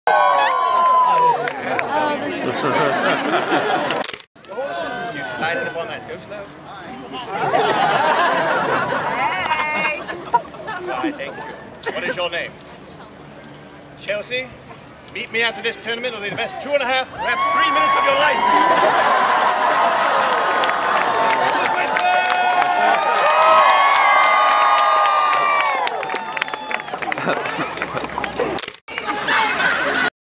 Ren Fest
That one time in my life when I got called for something, and it was at Ren Fest. SO GOOD. Life made. Sir Maximillion.